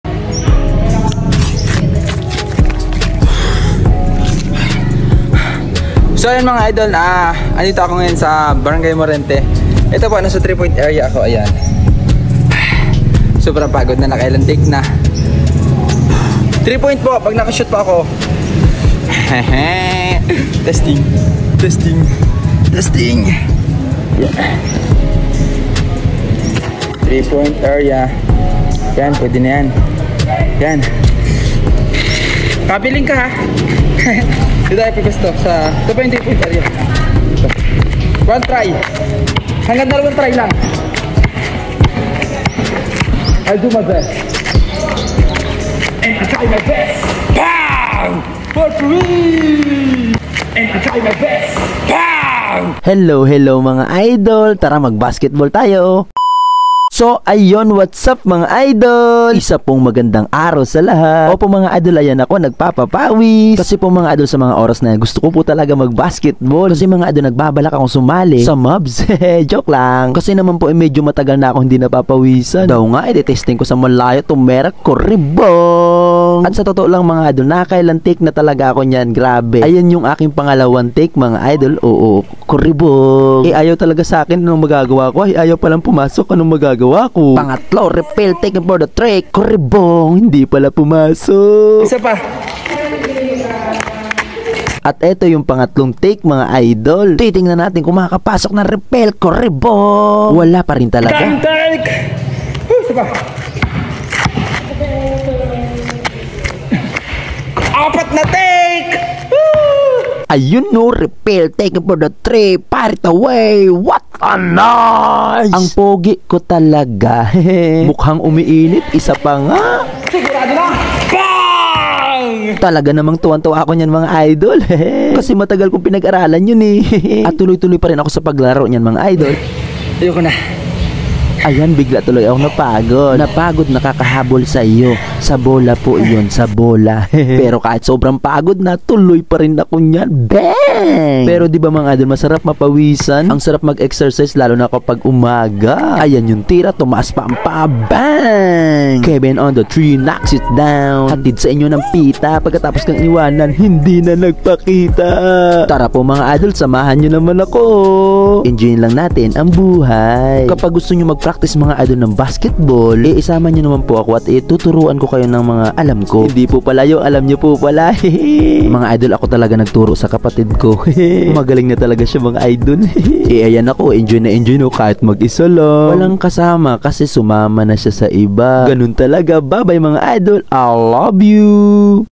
sobrang pagod pero kinaya ko mag basketball
tree point shot nakaya ko